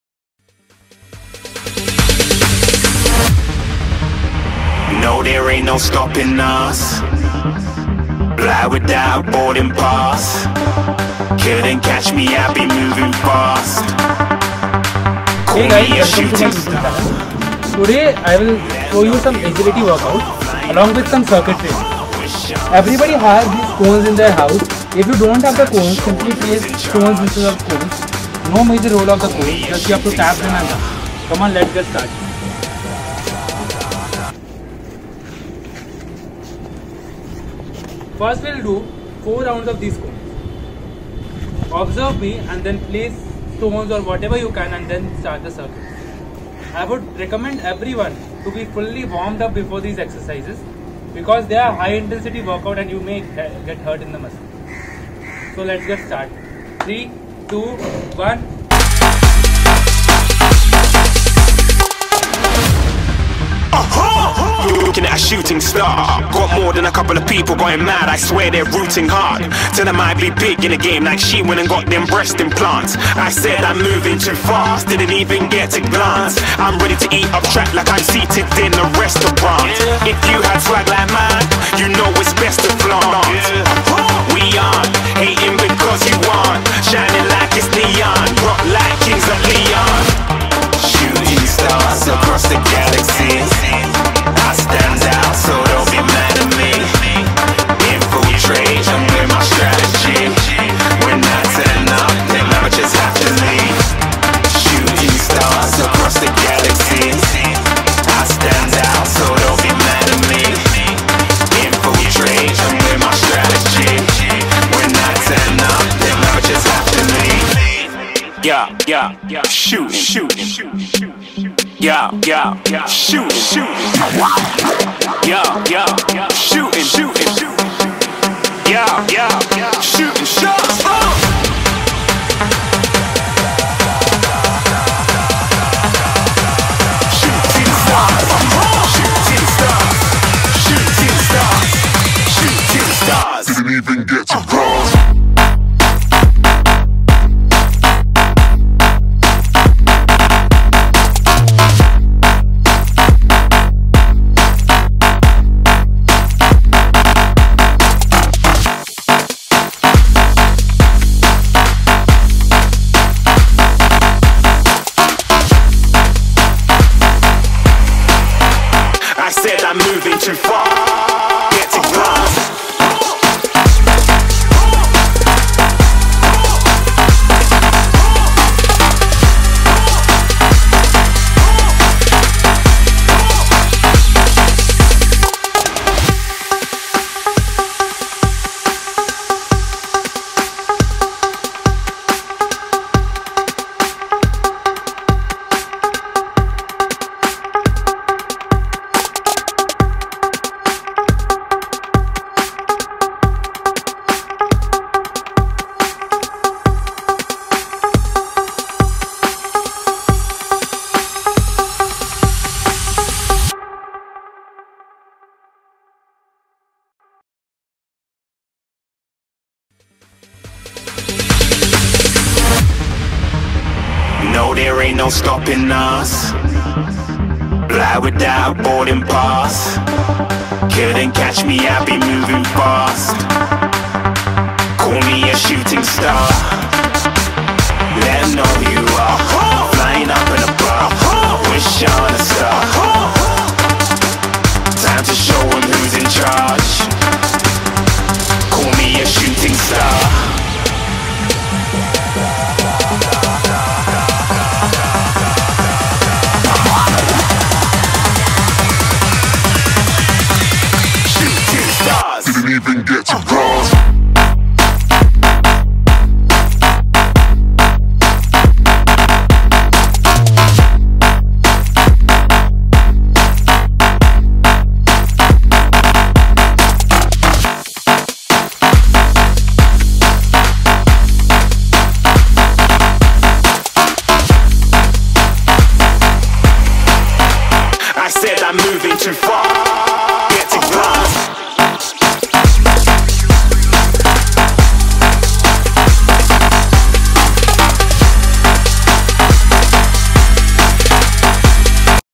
CIRCUIT TRAINING FOR WEIGHT LOSS AND BODY FITNESS 💪
This workout is the best conditioning workout any athlete can do during this period of time to maintain their level of stamina and endurance ...
If an athlete in doing this workout he is suggested to do this for a minimum of 2 rounds
If a beginner is doing this workout they are suggested to do this for a maximum of 1 round to start with.